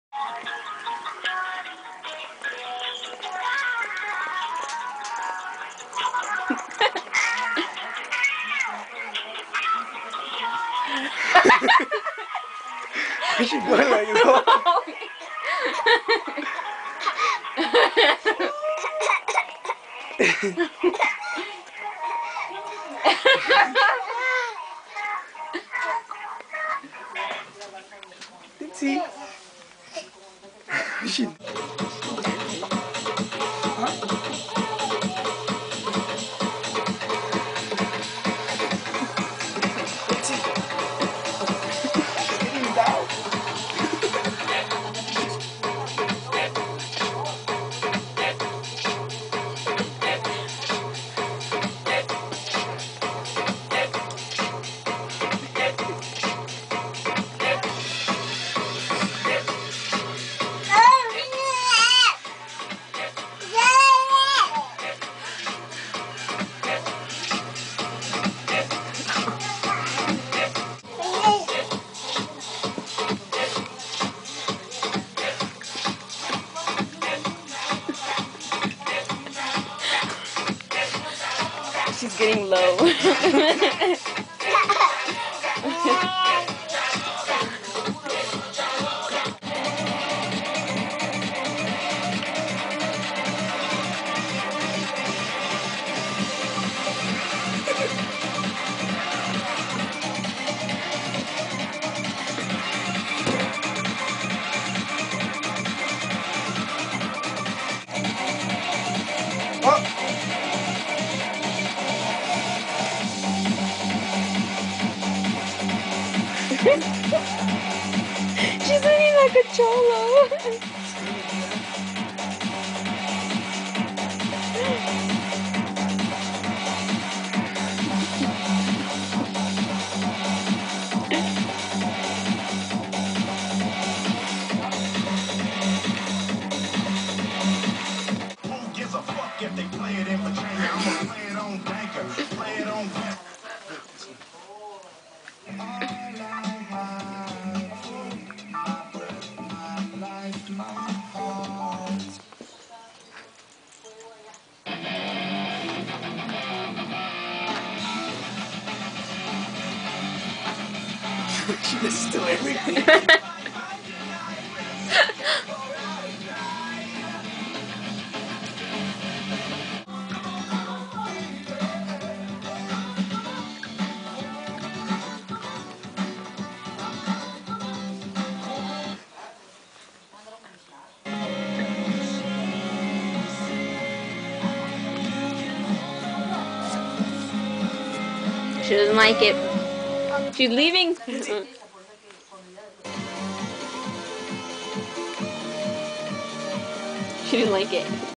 Baby dancing to Random Music
Baby Lindsay dancing to Random Music